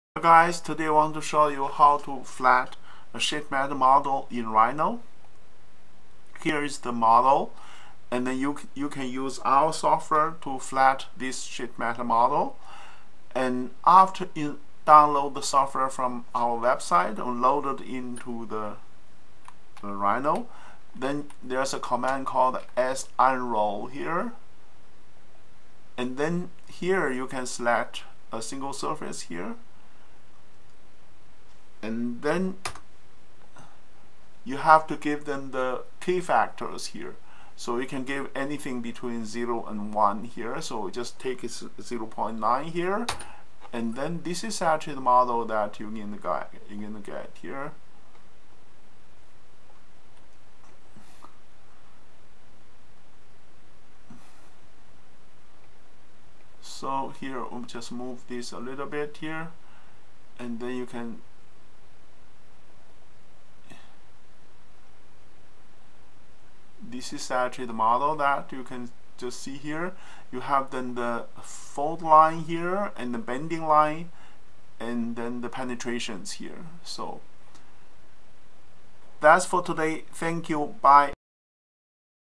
Flatten Sheet Metal Model inside Rhino
here is an example how to flatten a sheet metal model inside Rhino.